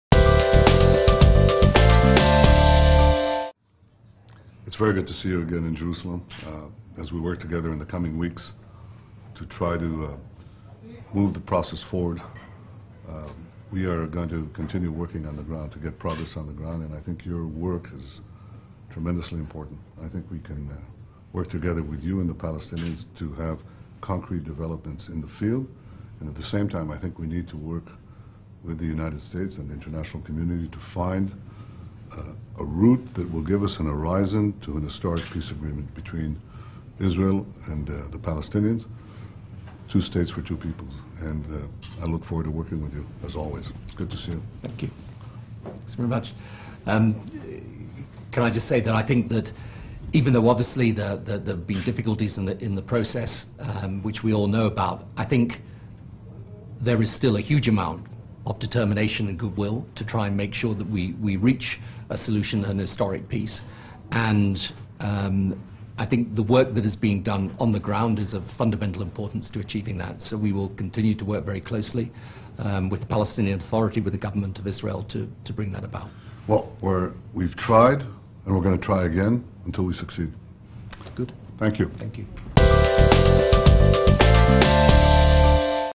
Netanyahu and Blair
PM Netanyahu meets with Quartet Special Envoy Tony Blair to discuss ways of working towards a historic peace agreement between Israel and the Palestinians, Dec. 9, 2010